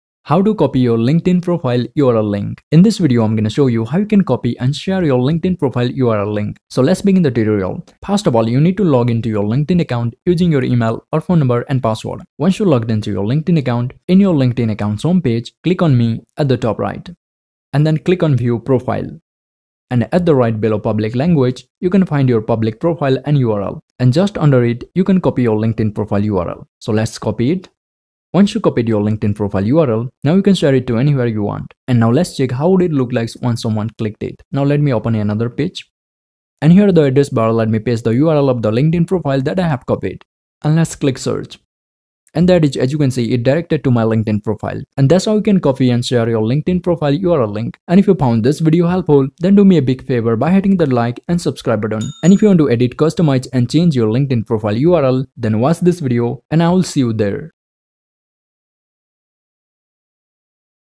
How to Copy Your LinkedIn Profile URL Link
How to Copy and Share LinkedIn Profile URL!

In this video you'll learn how to copy your profile url link on LinkedIn. So if you don't know how to find and copy your linkedin account's profile url, then this video is going to be very helpful for you. So make sure you watch this whole video and you can easily find, copy and share your linkedin profile url link.